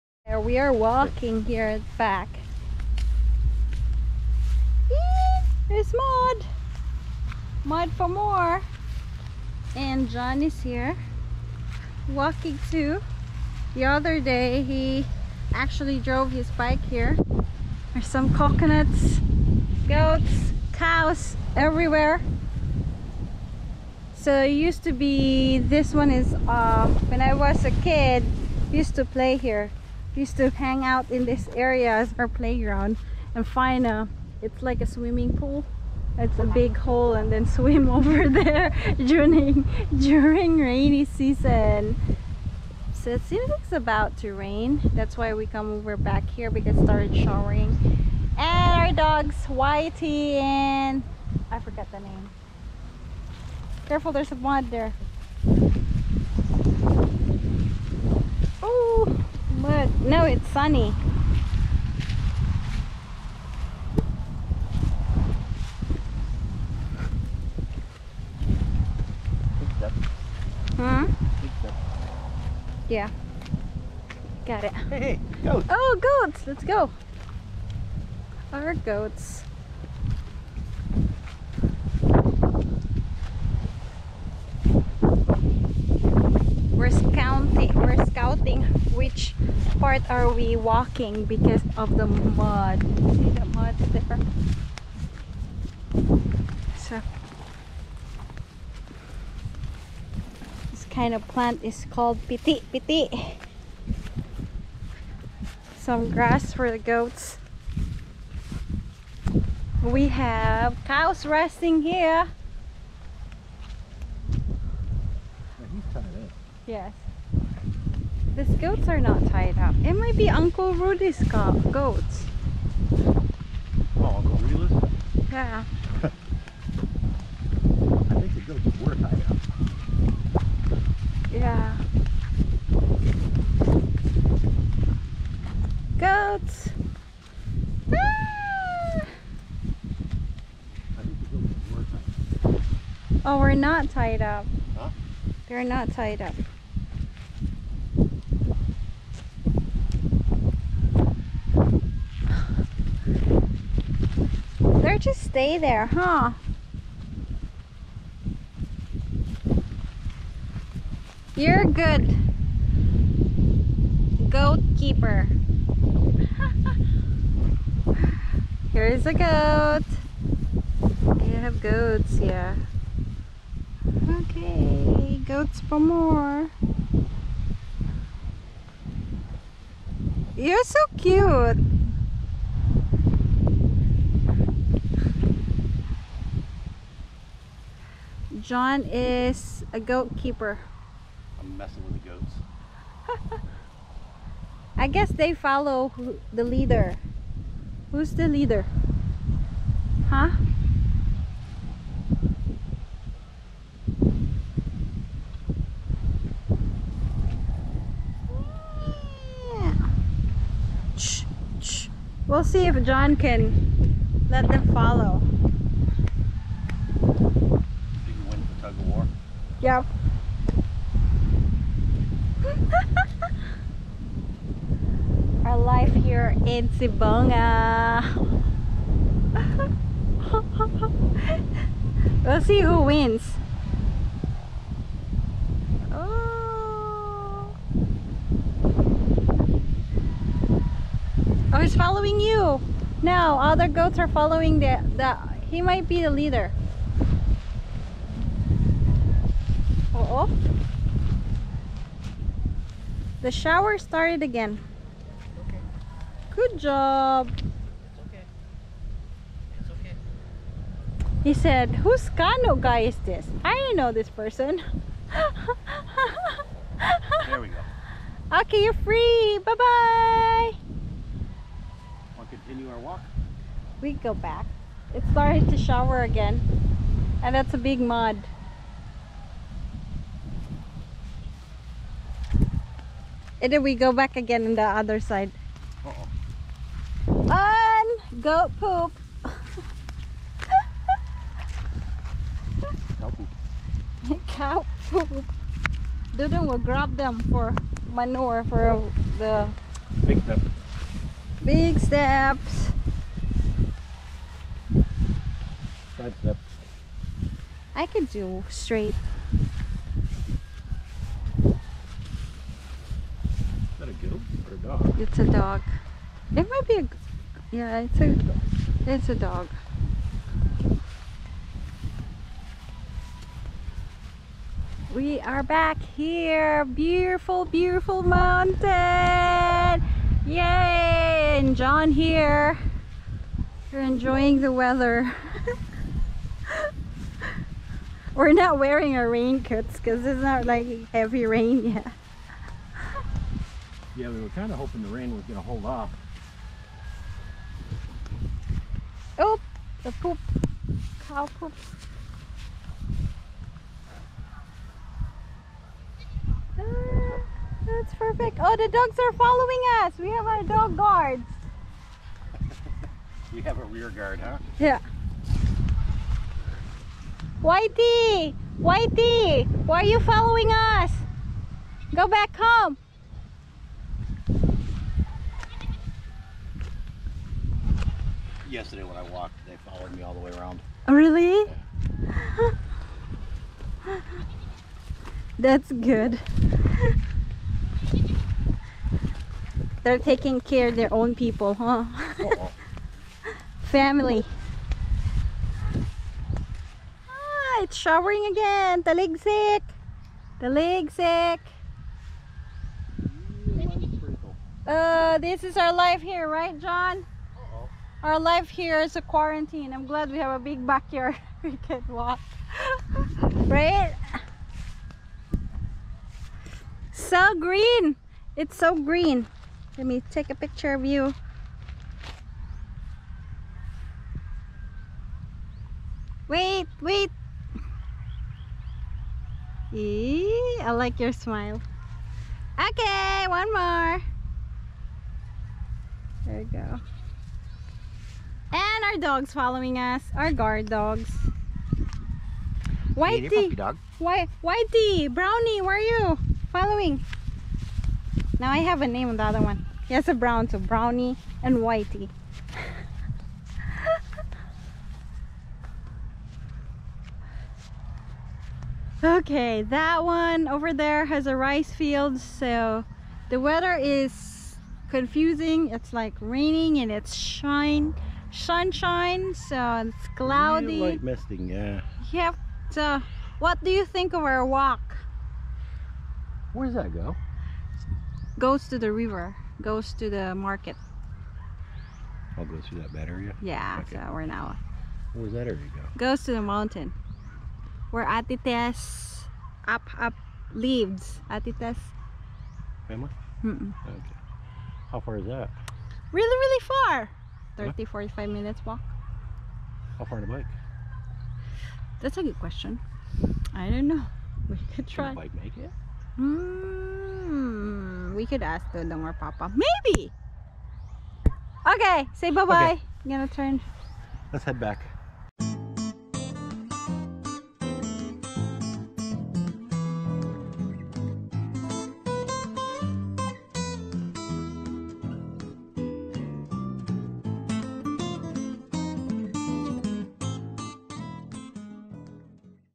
Province Life: Played with the goats (kanding), dogs (mga iro) and mud (lapuk)
Tried to do second round of our walk in the field and we saw the goats "kanding" (in Bisaya) and  played with it a little while and then the dogs "iro" (In Bisaya) keeps following us, Uncle Otik name it "Whitey and Brownie". Still we tried to scout if its best to ride the motorbike but it's still muddy "lapuk" (in Bisaya). So fun experience just wandering "suroy-suroy or laag-laag" in the field with the wild green grass "Pitik2x", seeing the cows "mga baka" in the shade resting while we continue walking in the mud and then seeing a banana "saging" trees bear fruits. And coconut "lubi" trees.

Indeed, this memories are very treasured and this season of life are worth it and very memorable for us. Just a simple walk and leaving the moment to be grateful to see green grass, trees, muds, goats, cows, dogs following us and field like this is worth it to savior. Totally, surreal and very different if you see it in a different perspective.

And will continue to share more on our experiences in the provinces.